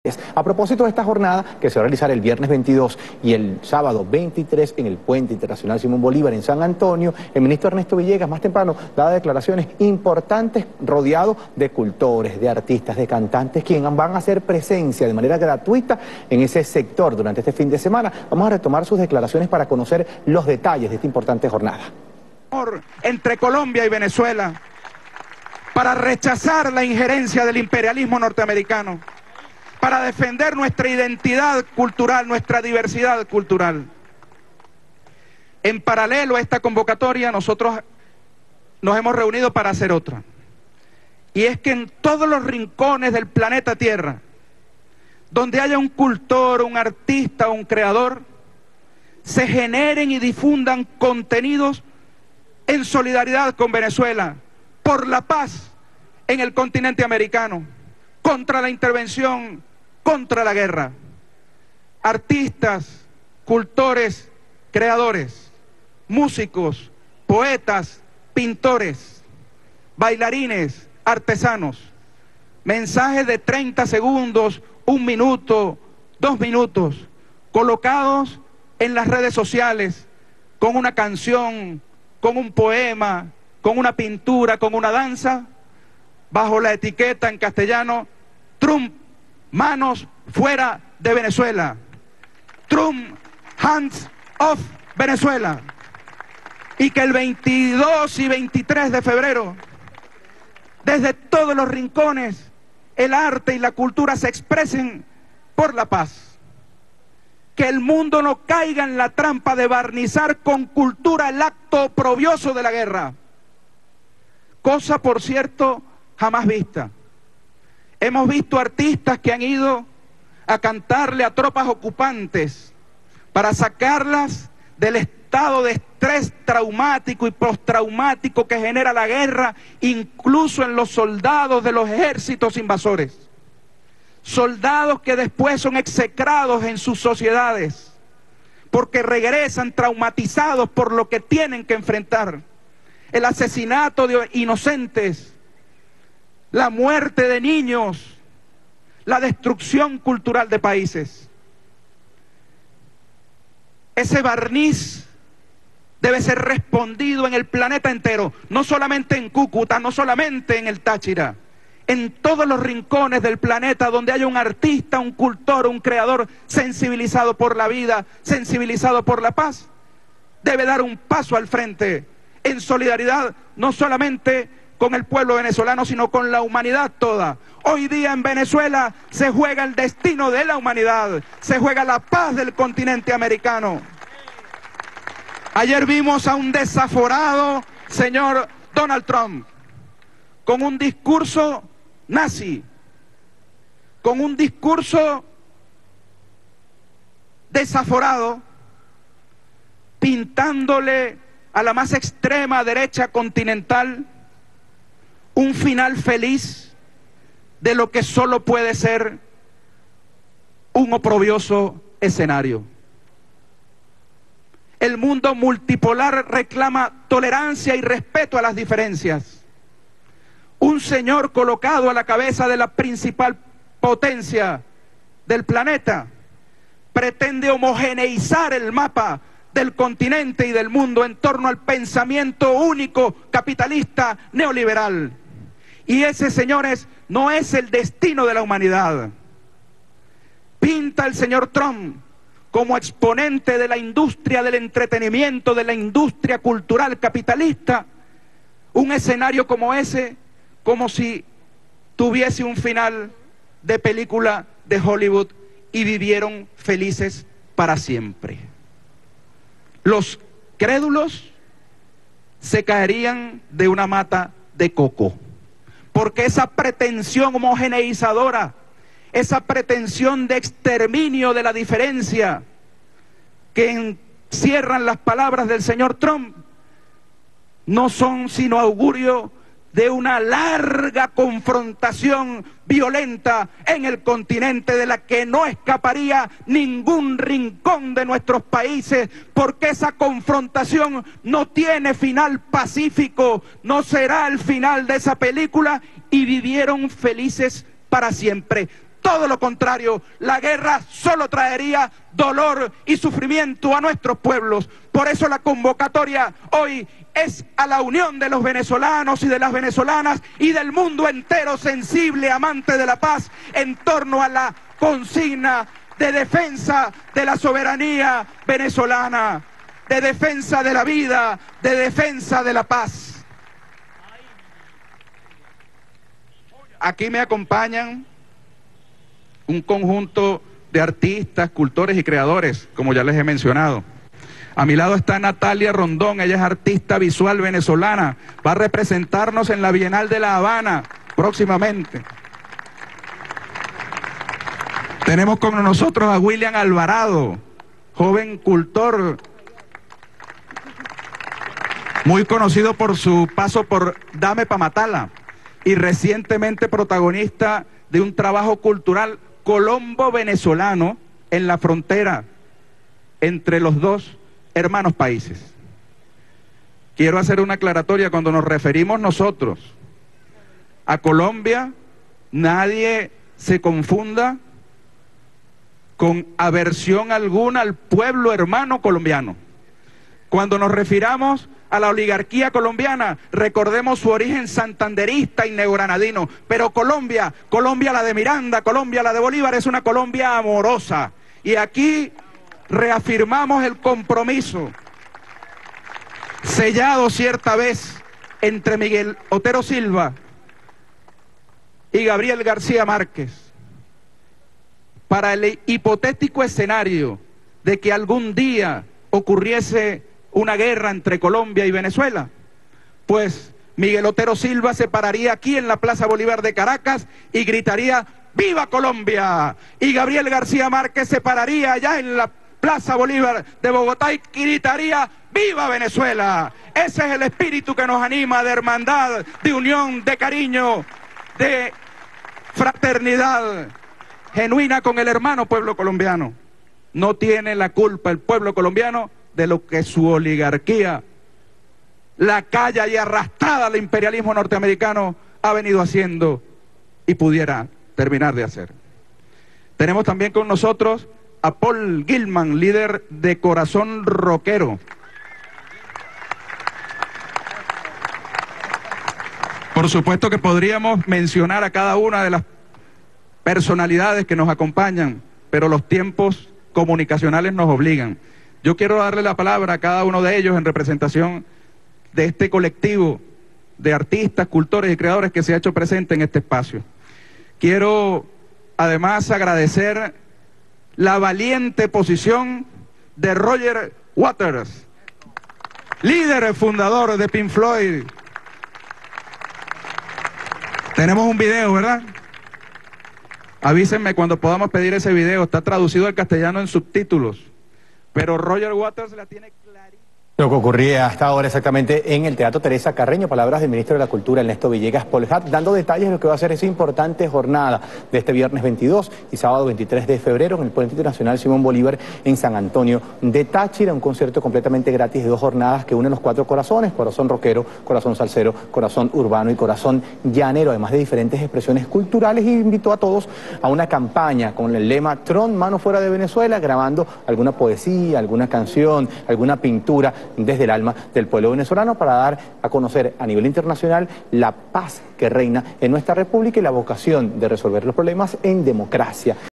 Ministro Ernesto Villegas convoca a artistas del mundo a campaña en apoyo a Venezuela
El ministro para la Cultura de Venezuela, Ernesto Villegas, convocó este 19 de febrero de 2019, en rueda de prensa desde el Teatro Teresa Carreño, a artistas del mundo a unirse a la Campaña Internacional de solidaridad con Venezuela ante las amenazas de intervención militar promovida por el gobierno de Estados Unidos. "De todos los rincones del planeta tierra, donde haya un cultor, un artista, invito a que se genere y difundan contenido en solidaridad con Venezuela, por la paz del continente americano, contra la intervención y contra la guerra", expresó Villegas en transmisión de Venezolana de Televisión.